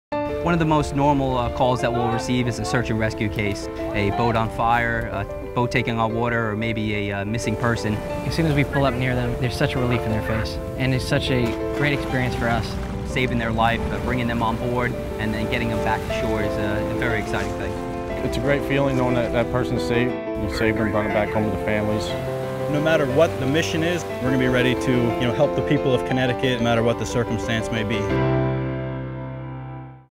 The United States Coast Guard in New Haven, CT
- The second 30 second promo featuring some of the Coast Guardsmen at Station New Haven. They talk about the importance on their job, and what it's like to save a life.